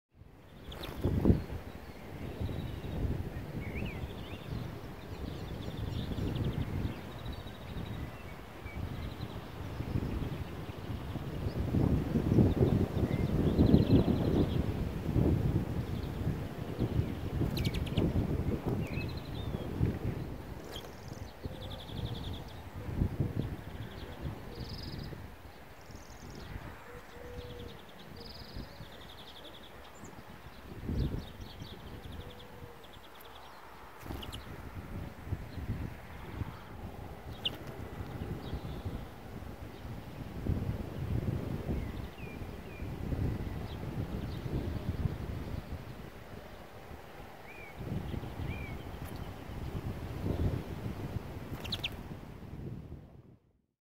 How do Greenfinches Drink? | Carduelis chloris | Cute Birds | Birdbath
One windy spring day, cute birds were drinking in my garden.
Don’t forget Water for Birds in all seasons!
Thank you very much and have a nice day!